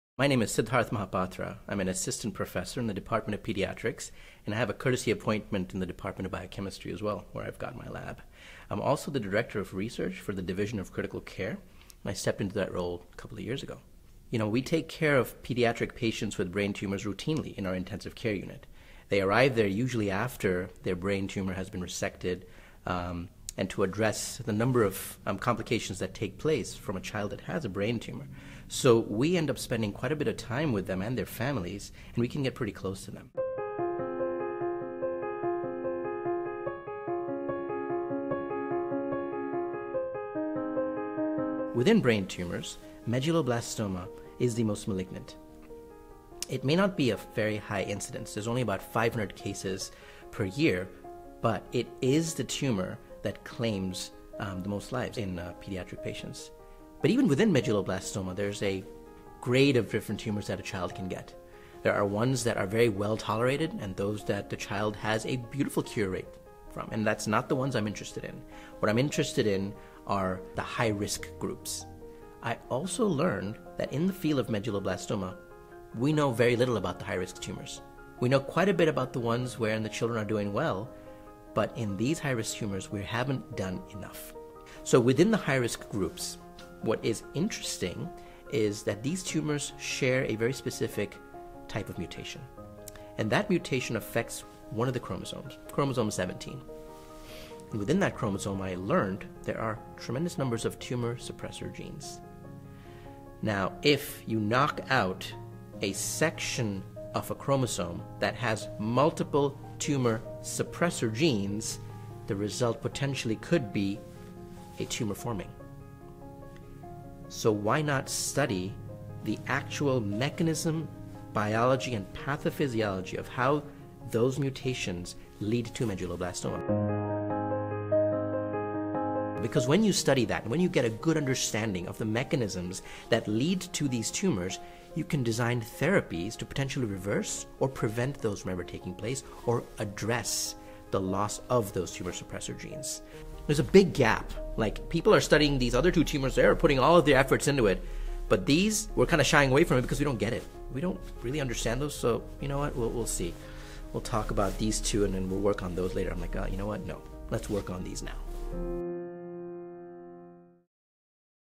Dr. Sidharth Mahapatra's High-Risk Medulloblastoma Project at UNMC
In March of 2020 the Team Jack Foundation announced grant funding for Dr. Sidharth Mahapatra's research project at the University of Nebraska Medical Center in Omaha, Nebraska. Dr. Mahapatra's project is titled chemosensitization of non-SHH/WNT medulloblastoma is regulated by miR-1253. This award is the first from funding raised through Team Jack's Power5 Pediatric Brain Tumor Initiative, which is a pledge to fund $5 million in pediatric brain cancer research over ten years at UNMC. To learn more about this project and all of the Team Jack Foundation funded research